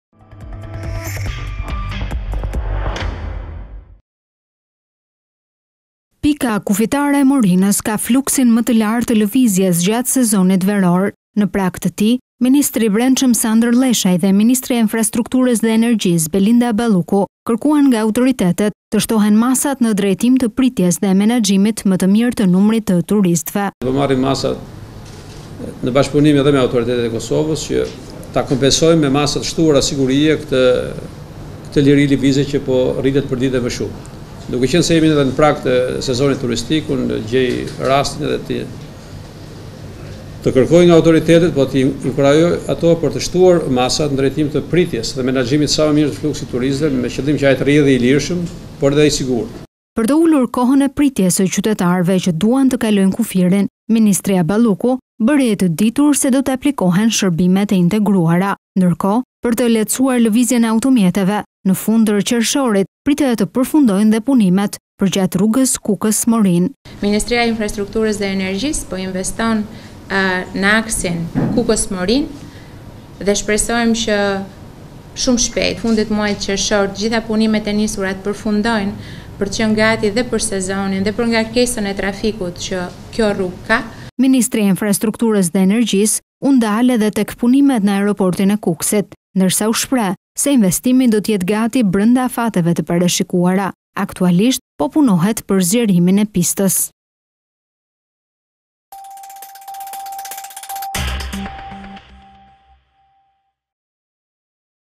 Sezoni veror, Balluku dhe Lleshaj ne Morine| ABC News Albania
Strukturat shtetërore do të jenë të angazhuara për të përballuar fluksin e turistëve që do të vizitojnë vendin tonë gjatë sezonit veror. Ministri i Brendshëm Lleshaj dhe Ministrja e Infrastrukturës dhe Energjisë Balluku, e dhanë këtë garanci nga pika kufitare e Morinës.

Pika kufitare e Morinës ka fluksin më të lartë të lëvizjes gjatë sezonit veror.  Në prag të tij ministri i Brendshëm Sandër Lleshaj dhe ministrja e Infrastrukturës dhe Energjisë Belinda Balluku kërkuan nga autoritetet të shtohen masat në drejtim të pritjes dhe menaxhimit më të mirë të numrit të turistëve.

“Do të marrim masa në bashkëpunim edhe me autoritetet e Kosovës që ta kompensojmë me masa të shtuara sigurie këtë liri të lëvizjes, që po rritet përditë e më shumë. Duke qenë se jemi edhe në prag të sezonit turistik, unë gjej rastin të kërkoj nga autoritetet dhe t’i inkurajoj ato, për të shtuar masat në drejtim të pritjes dhe menaxhimit sa më të mirë të fluksit të turistëve, me qëllim që ai të rrjedhë i lirshëm por edhe i sigurt. Të shtohet ato masa që tradicionalisht janë ndërmarrë për patrullimin e përbashkët, për kontrollin e përbashkët e çështje të tjera”.

Për të ulur kohën e pritjes së qytetarëve që duan të kalojnë kufirin ministrja Balluku bëri të ditur se do te aplikohen shërbimet e integruara.

Ndërkohë për të lehtësuar lëvizjen e automjeteve në fund të Qershorit pritet të përfundojnë dhe punimet përgjatë rrugës Kukës –Morinë.

 “Fokusi kryesor në zonën e Kukësit është infrastruktura. Ministria e Infrastrukturës dhe Energjisë po investon në aksin Kukës-Morinë dhe shpresojmë që shumë shpejt, brenda fundit të muajit qershor, të gjitha punimet e nisura të përfundojnë për të qenë gati dhe për sezonin dhe për ngarkesën e trafikut që kjo rrugë ka”.

Ministrja e Infrastrukturës dhe Energjisë u ndal edhe punimet në aeroportin e Kukësit ndërsa u shpreh se investimi do jetë gati brenda afateve të parashikuara. Aktualisht po punohet për zgjerimin e pistës./abcnews.al